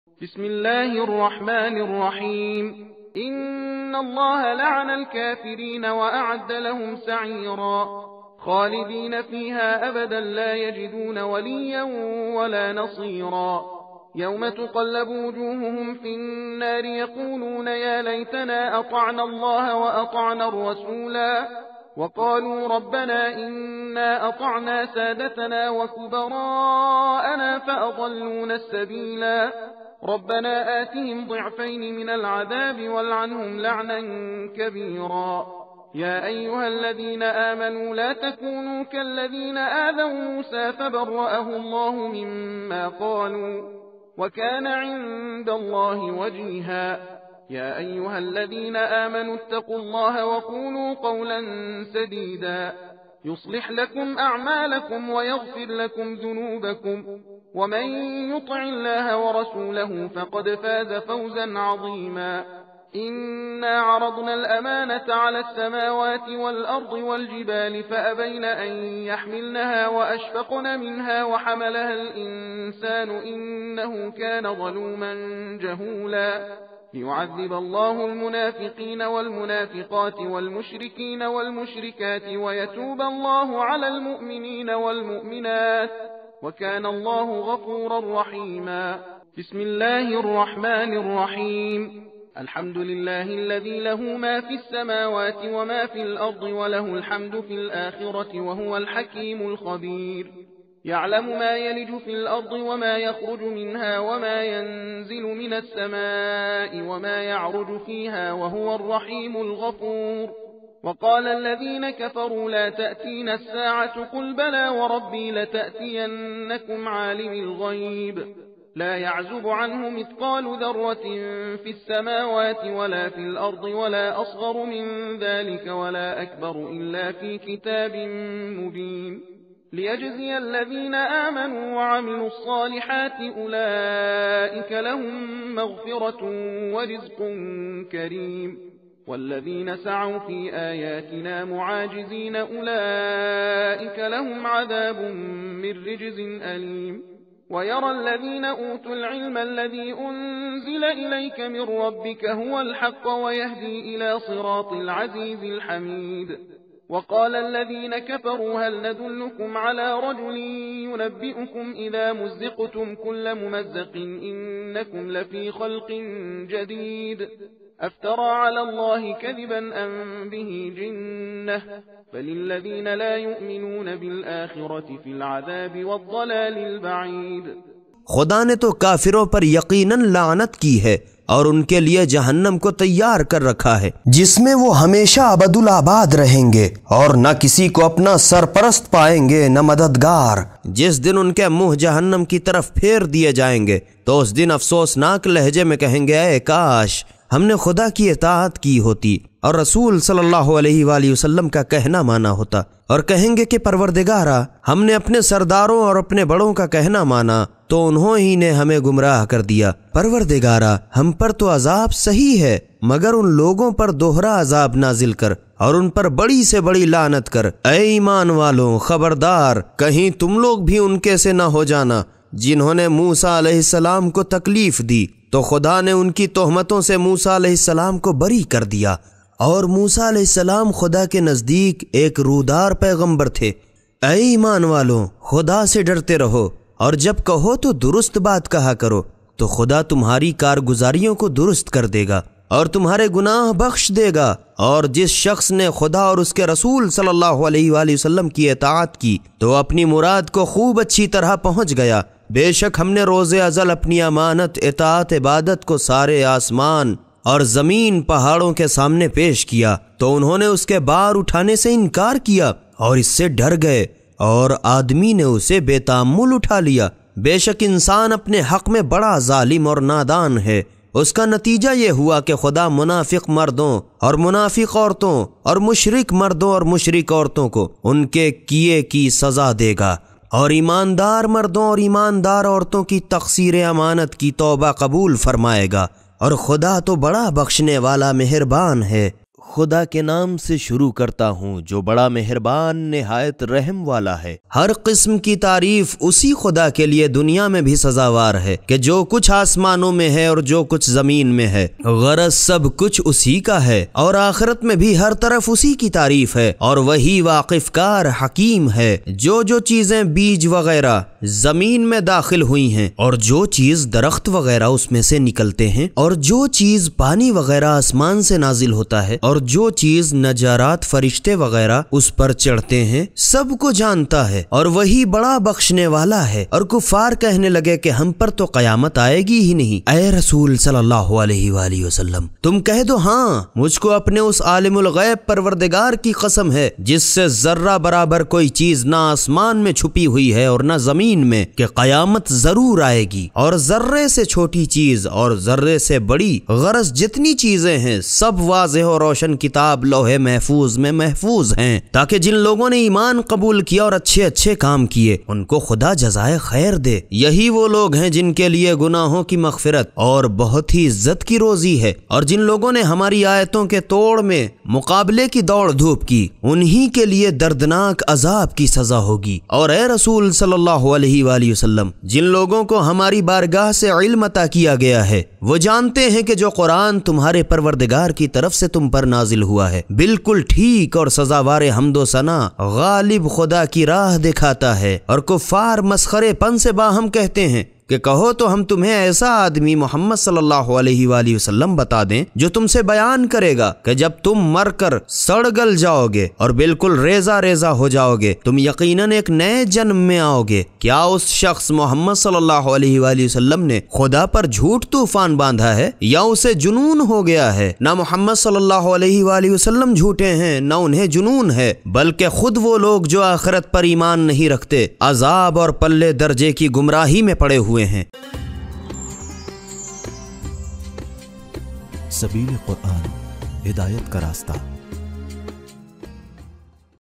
Para 22/30 | 7 Min with Quran | Day 223 | Tilawat with Tarjuma | Quran Urdu Tarjuma
Reciter: Aagha Motaz Aaghai
Translator: M Farman Ali sb
Narrator: M Farzan sb

Support Us
Islamic Knowledge ko Nashr Karne Ke Liye Hamein Support Karen!!!

Bank Name:  ICICI BANK LIMITED
Account Holder Name:  Sabeel Welfare Foundation